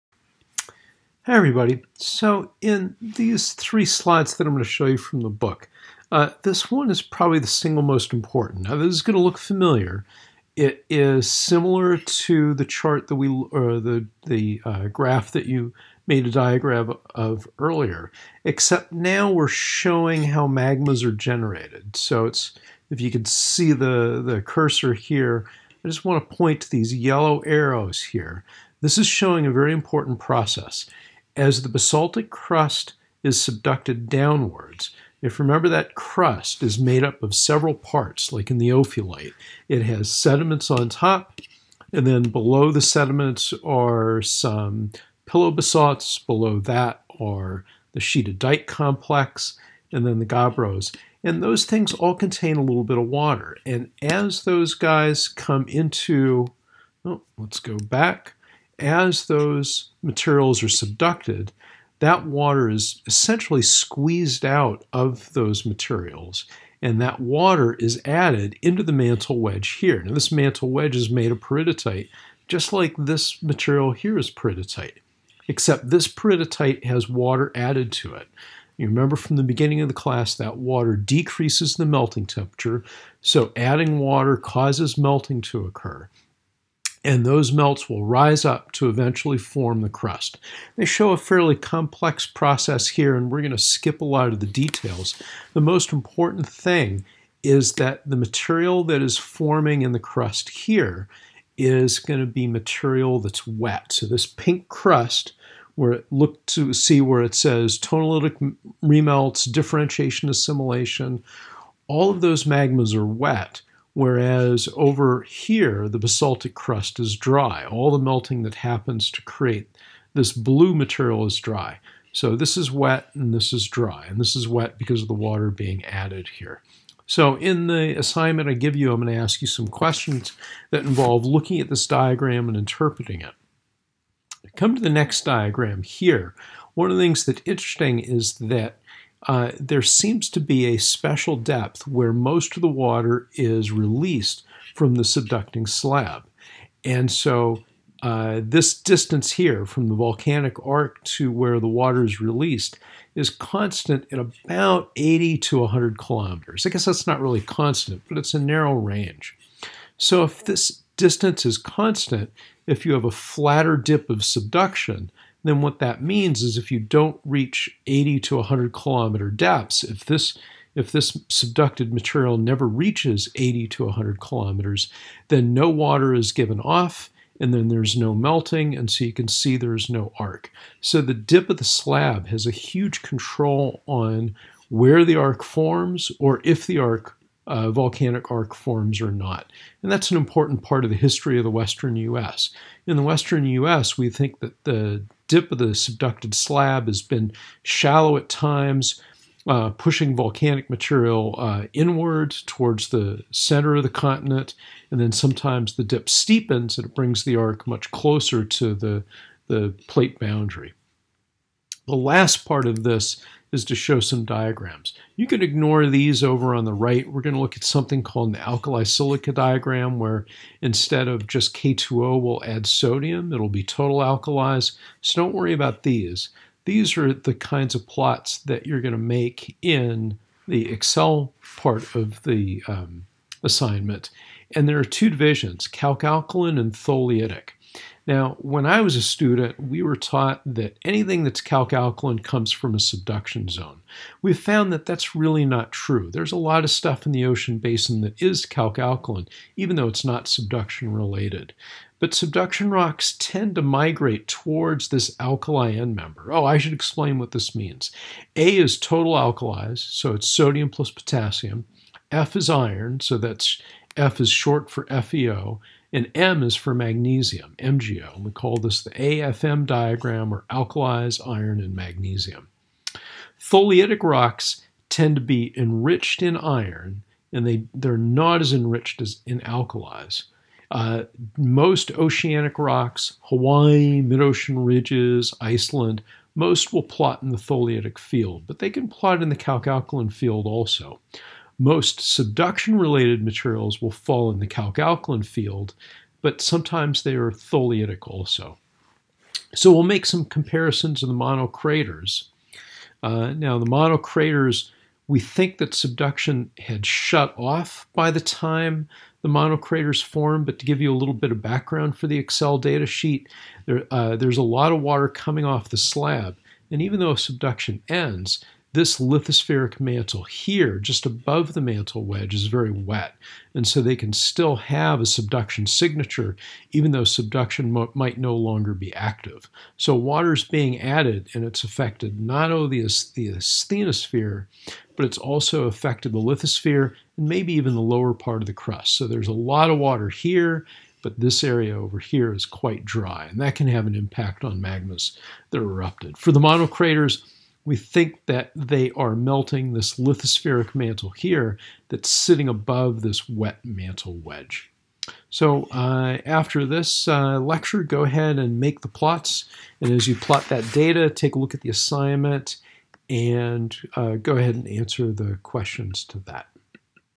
Subduction-related volcanism
Uses figures from John Winter's text to show how magmatism and volcanic activity are generated at subduction zones (by Keith Putirka)